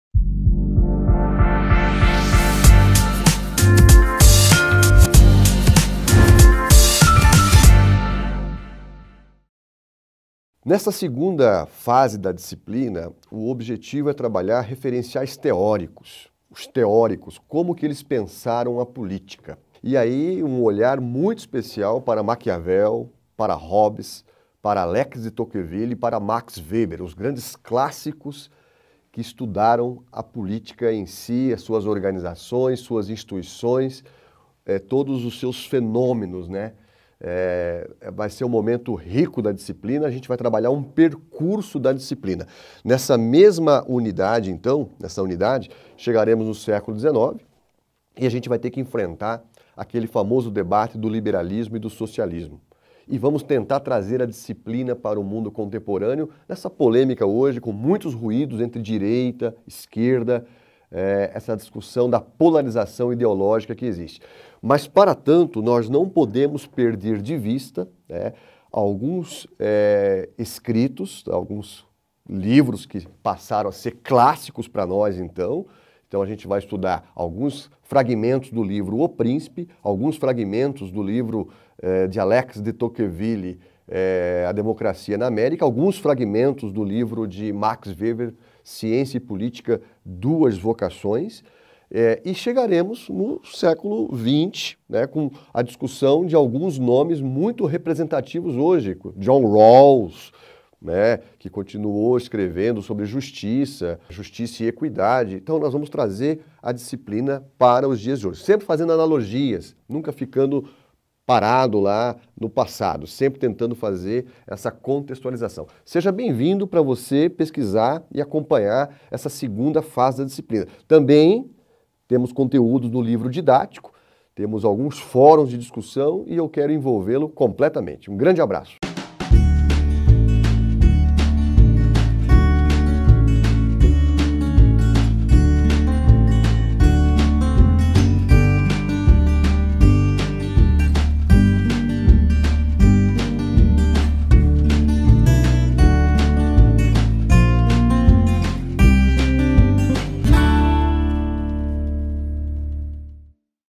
[LIBRAS] Referenciais Teóricos da Ciência Politica
Interpretação em Libras -  Referenciais Teóricos da Ciência Politica na disciplina de Ciência Politica com o professor Cláudio Cesar de Andrade para o curso de Graduação em Tecnologia em Gestão Pública.

Intérprete de Libras: Professora Ana Paula Silva